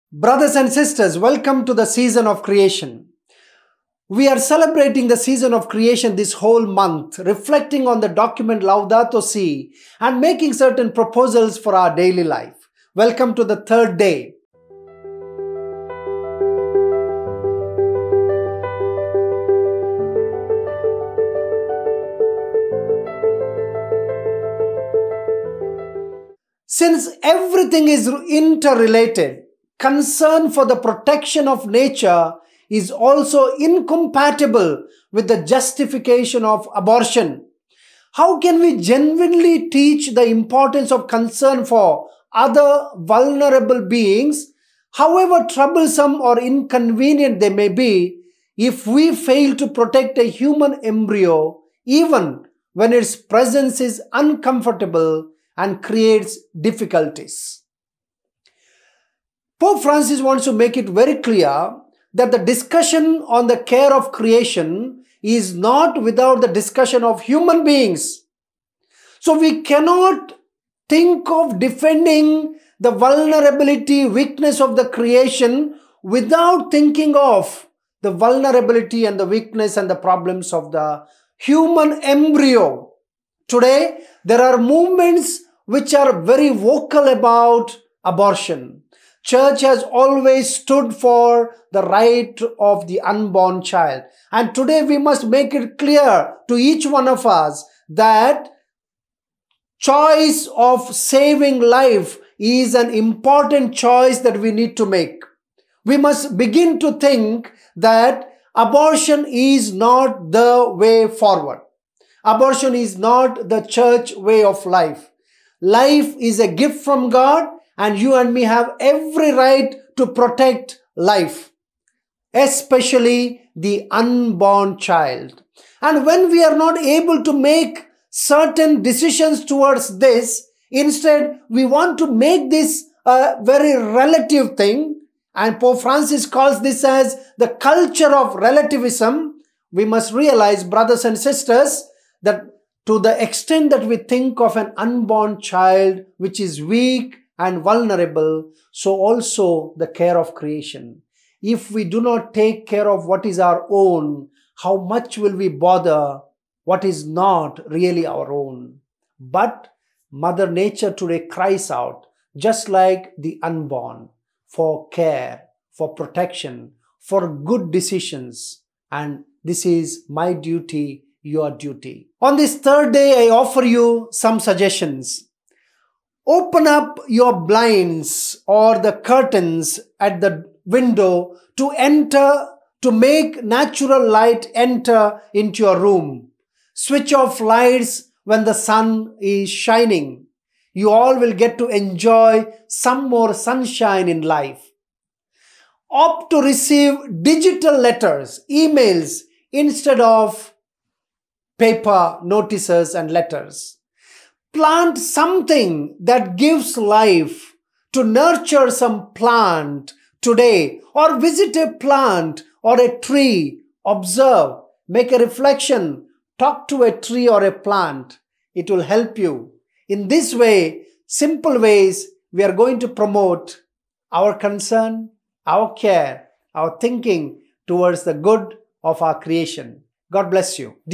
SeasonofCreation2020 | 1 Sep- 4 Oct | Thoughts on LaudatoSi | Fr. Praveen D Souza, OFM | 3 Sep
The Season of Creation is a time to renew our relationship with our Creator and all creation through celebration, conversion, and commitment together. During the Season of Creation, we join our sisters and brothers in the ecumenical family in prayer and action for our common home.

Ecumenical Patriarch Dimitrios I proclaimed 1 September as a day of prayer for creation for the Orthodox in 1989. In fact, the Orthodox church year starts on that day with a commemoration of how God created the world. The World Council of Churches was instrumental in making the special time a season, extending the celebration from 1 September until 4 October. Pope Francis made the Roman Catholic Church’s warm welcoming of the season official in 2015.

In recent years, statements from religious leaders around the world have also encouraged the faithful to take time to care for creation during the month-long celebration. The season starts 1 September, the Day of Prayer for Creation, and ends 4 October, the Feast of St. Francis of Assisi, the patron saint of ecology beloved by many Christian denominations
Courtesy: